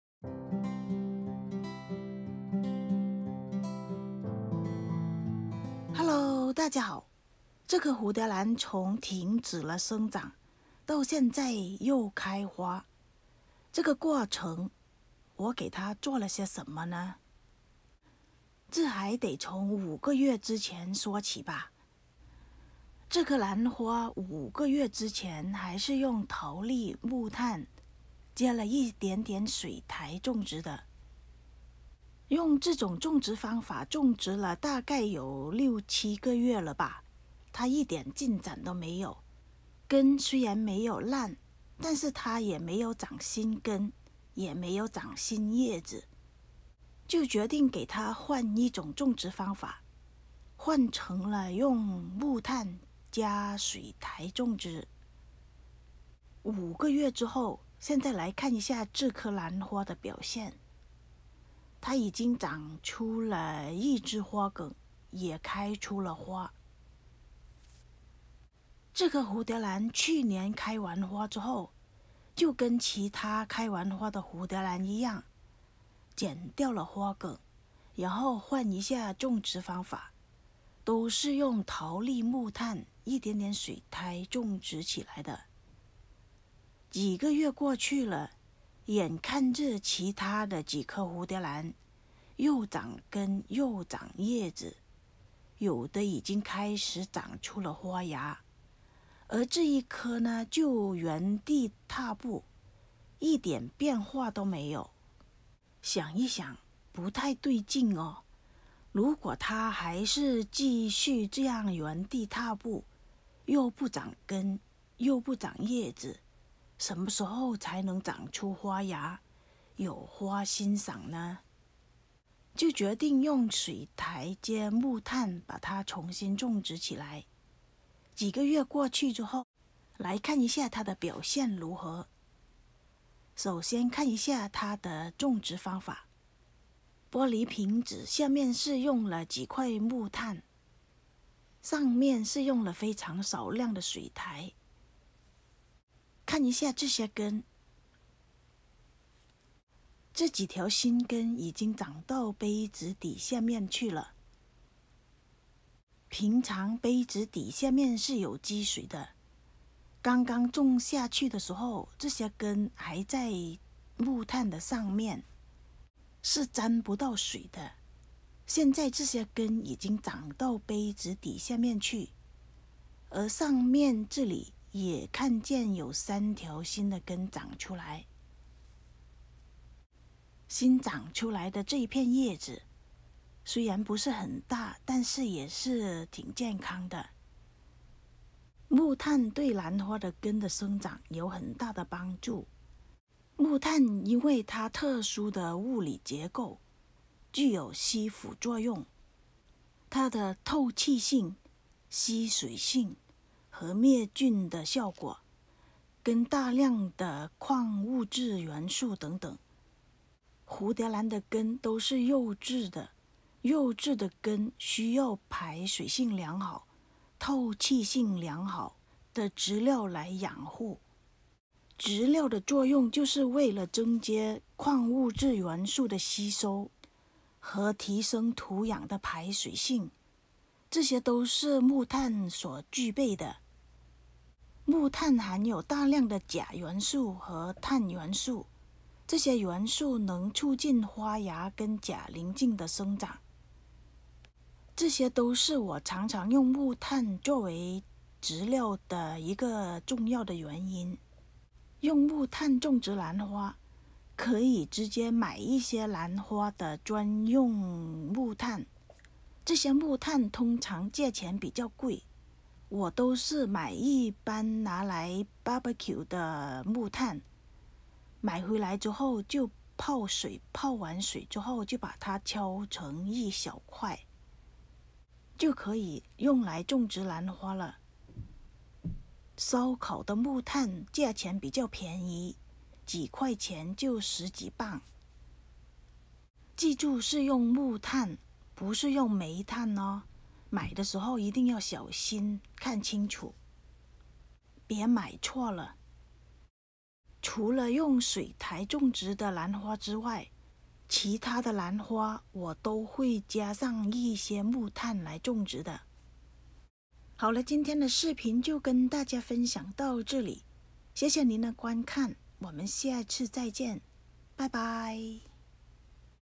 07/16/2022 曾经停止生长，现在兰花朵朵。木炭起到怎样的作用？The effect of charcoal on Phalaenopsis
The effect of charcoal on Phalaenopsis